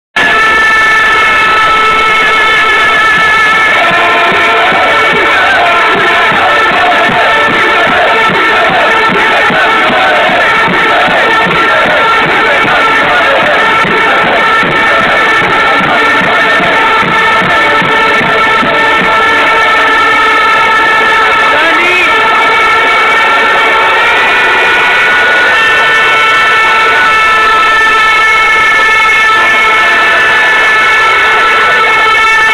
Oltchim intra la incalzire in uralele spectatorilor
incalzirea de dinaintea calificarii in semifinale